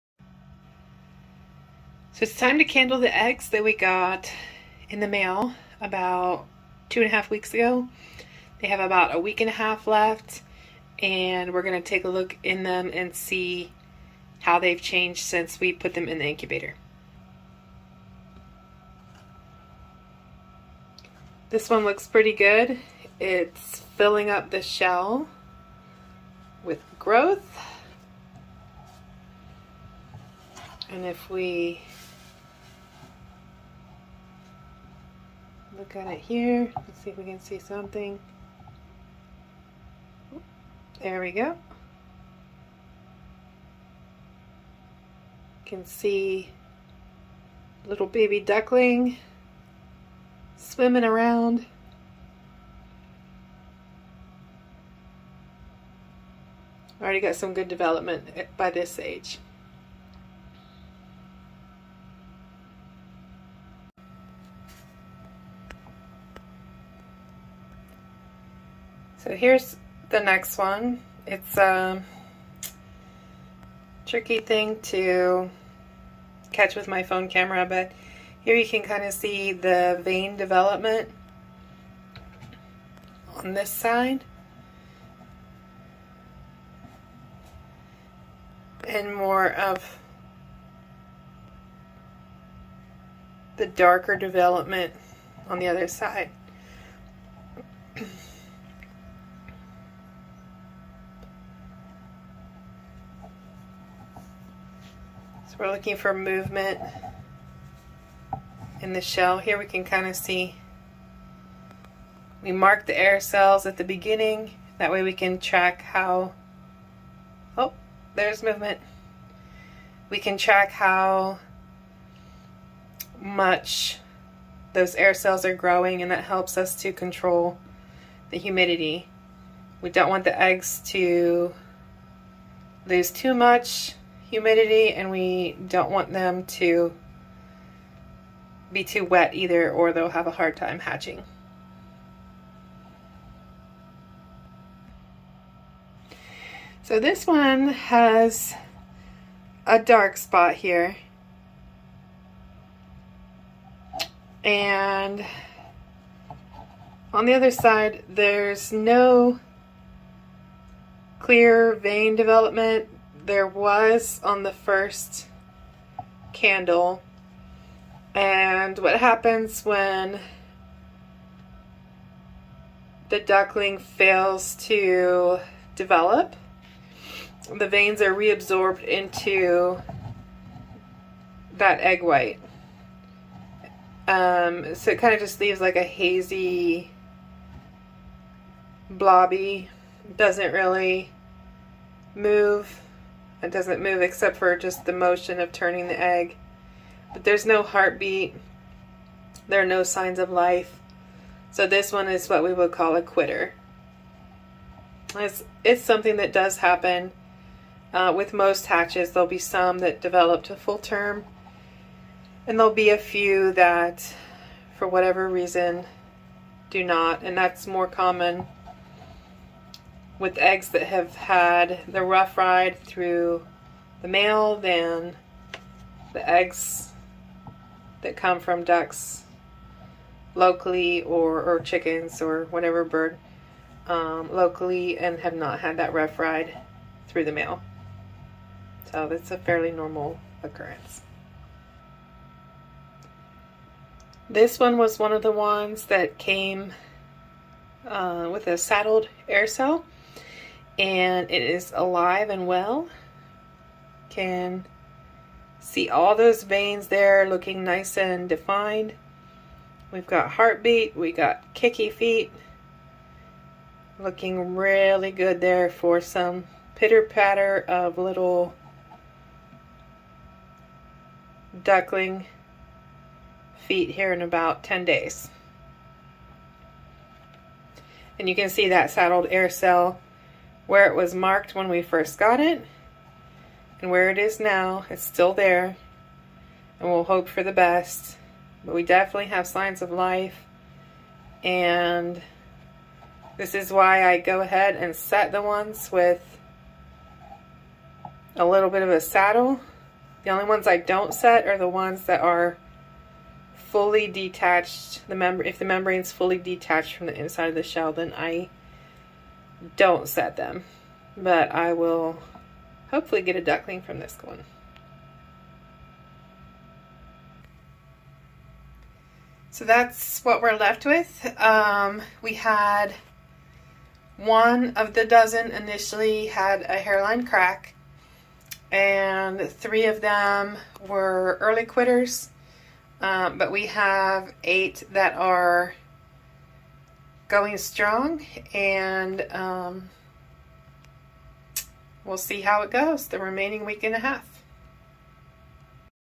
Hatching Welsh Harlequin Ducks - Candling the Eggs on Day 18
Hatching Welsh Harlequin Ducks
Day 18 of a 28 Incubation Period